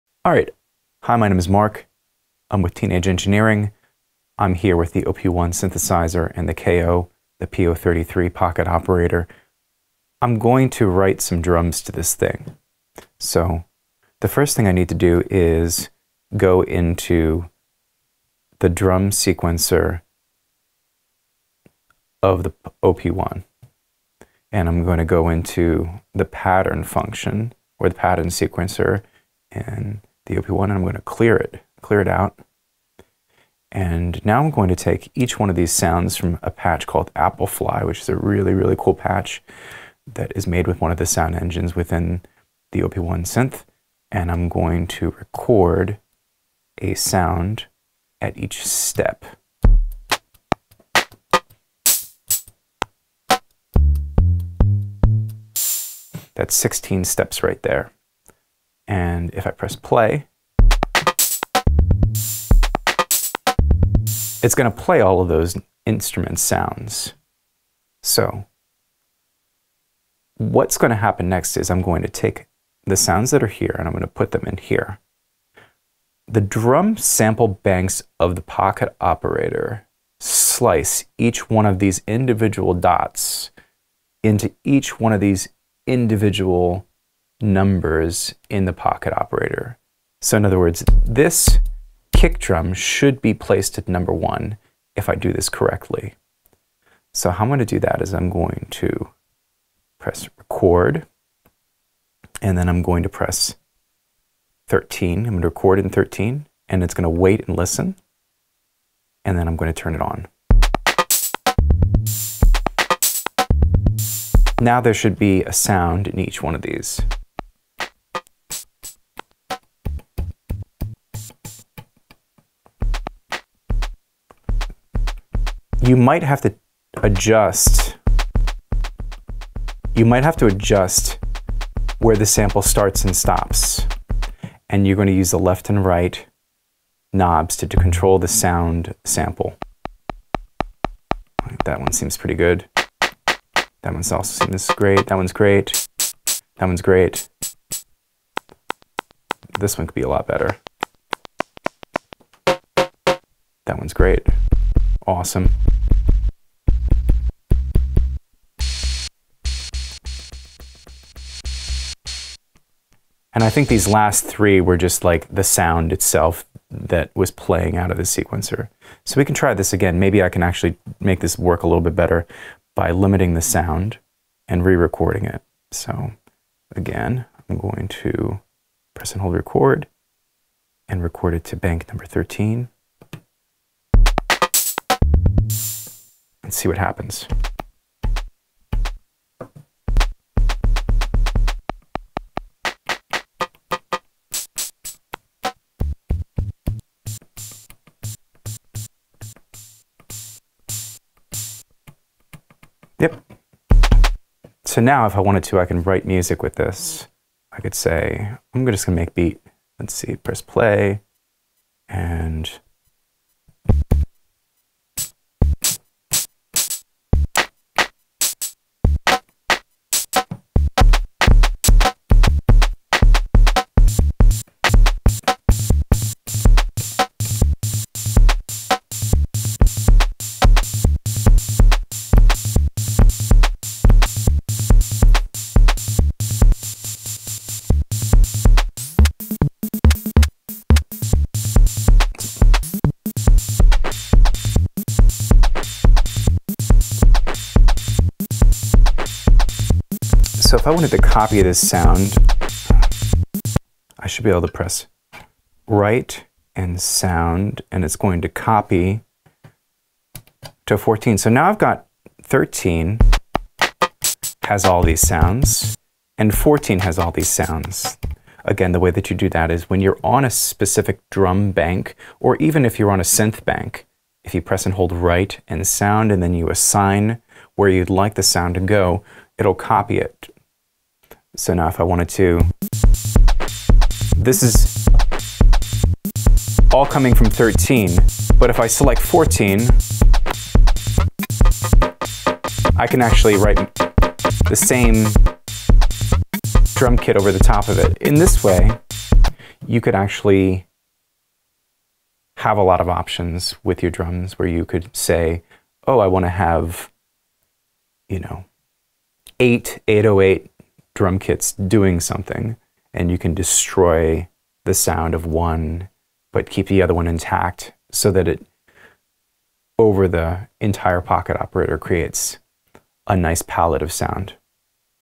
Teenage Engineering PO-33 K.O. Sampling Drums from the OP-1
Sample drums from your OP-1 on your TE PO-33 K.O. Mark DeNardo walks us through the communication process between the OP-1 and the Pocket Operator.